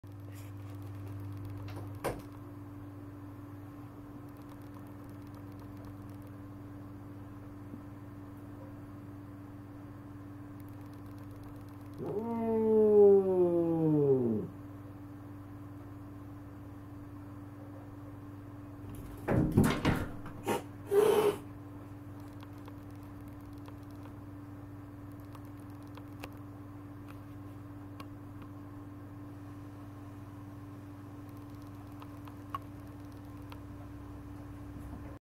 Cat jousting
Two cats separated by glass, still attempt to fight!
Oh No! I have become a maker of funny cat videos! Help! Help! What do I do now?